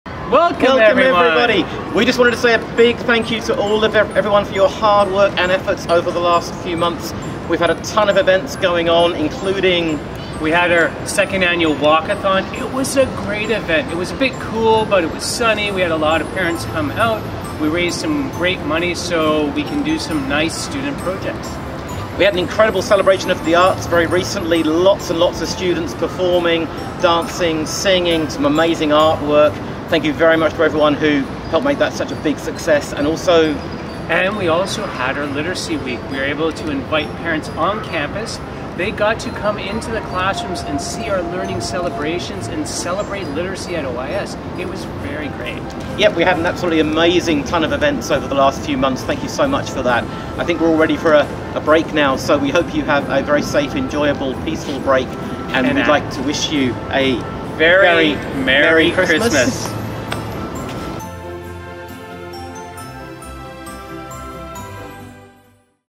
Principals End of Year Message 2023
Principals Dwayne Primeau and Mark Beales close out 2023 with a message to the OYIS Community.